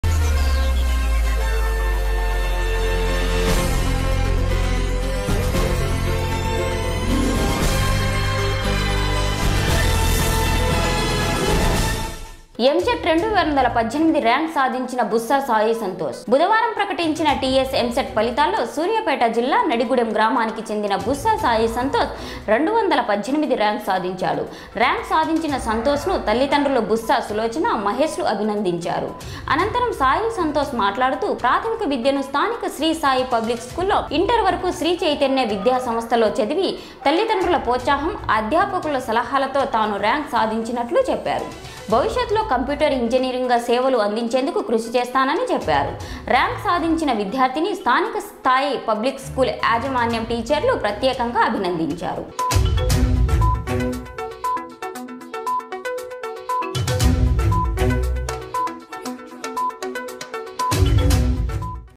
ఎంసెట్ లో 218 ర్యాంక్ సాధించిన బుస్సా సాయి సంతోష్
బుధవారం ప్రకటించిన TS ఎంసెట్ ఫలితాల్లో సూర్యాపేట జిల్లా నడిగూడెం గ్రామానికి చెందిన బుస్సా సాయి సంతోష్ 218 ర్యాంక్ సాధించాడు. ర్యాంక్ సాధించిన సంతోష్ ను తల్లిదండ్రులు బుస్సా సులోచన,మహేష్ లు అభినందించారు. అనంతరం సాయి సంతోష్ మాట్లాడుతూ. ప్రాథమిక విద్యను స్థానిక శ్రీ సాయి పబ్లిక్ స్కూల్లో, ఇంటర్ వరకు శ్రీ చైతన్య విద్య సంస్థల్లో చదివి తల్లిదండ్రుల ప్రోత్సహం, అధ్యాపకుల సలహాలతో తాను ర్యాంక్ సాధించినట్లు చెప్పారు. భవిష్యత్ లో కంప్యూటర్ ఇంజినీర్ గా సేవలు అందించేందుకు కృషి చేస్తానని చెప్పారు. ర్యాంక్ సాధించిన విద్యార్థిని స్థానిక సాయి పబ్లిక్ స్కూల్ యాజమాన్యం , టీచర్లు, ప్రత్యేకంగా అభినందించారు..